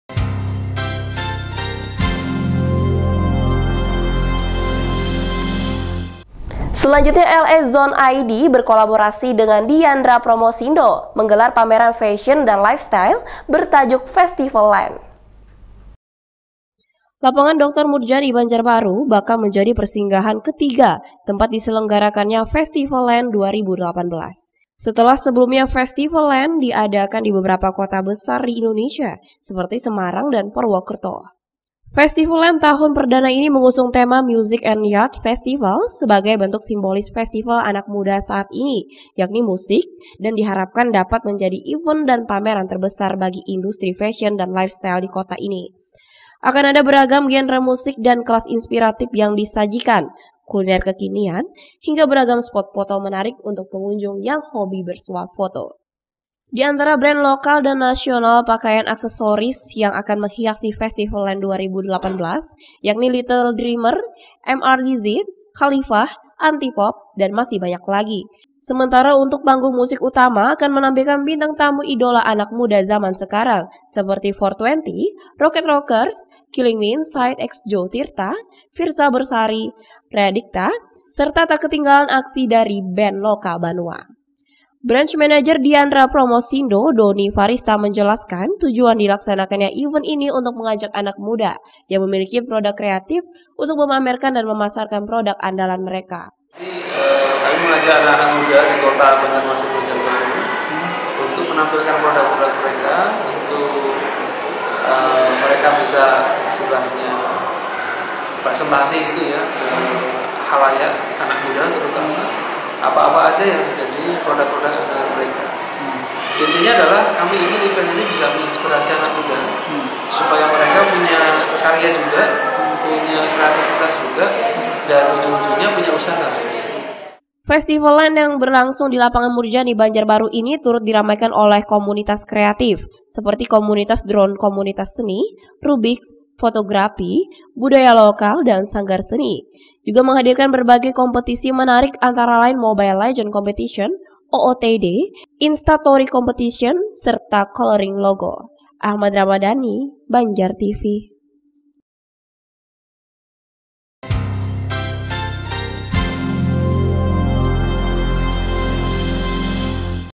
FESTIVALAND HADIR DI KALSEL AJANG PRODUK KREATIF DAN HIBURAN ANAK MUDA
LA Zone ID, berkolaborasi dengan Dyandra Promosindo, menggelar pameran fashion dan lifestyle, bertajuk Festivaland.